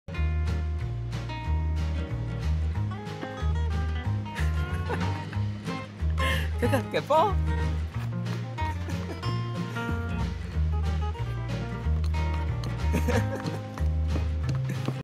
BEST CORGI mini GOLF attraction
Cocoa the Corgi plays mini golf in a variety of ways. Hole in one? (B)Eagle? Corgie?

Enjoy Cocoa's footage!